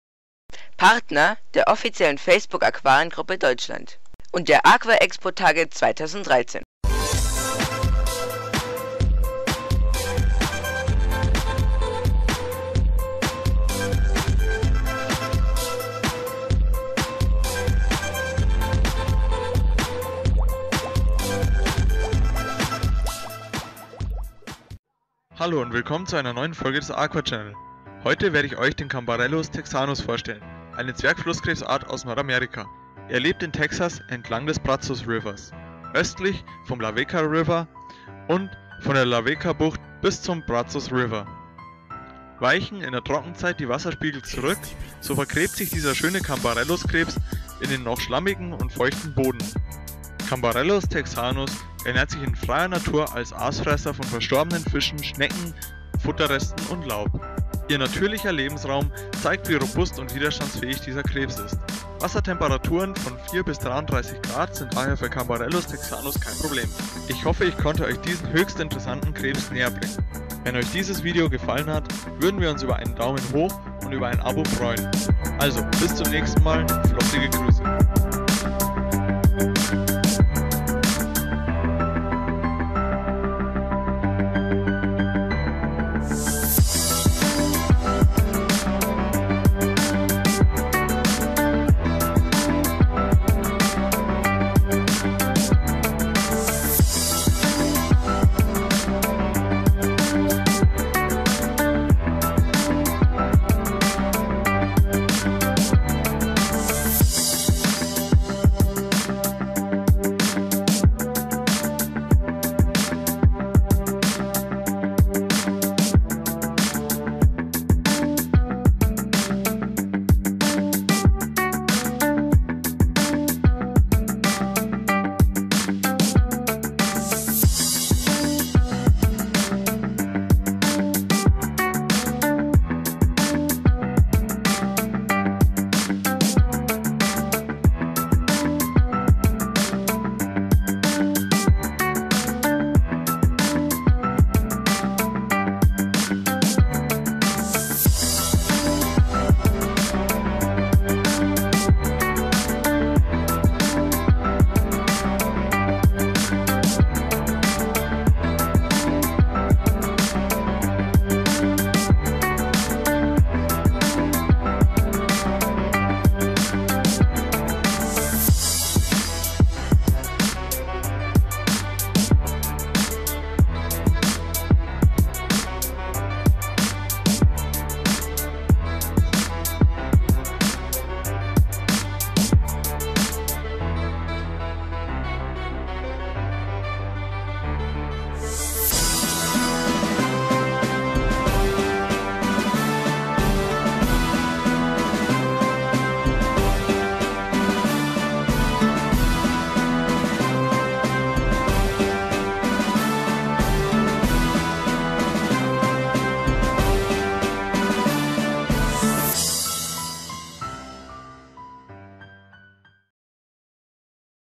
Cambarellus Texanus
In diesem Video stellt euch Christoph seinen neuen Mitbewohner aus Texas, den Cambarellus Texanus, vor. Dieser äußerst schön gezeichnete Flusskrebs kommt in Flusssystemen in Texas vor.

Ein Video des AquaChannel, 2013
Produziert von Christoph Weinberger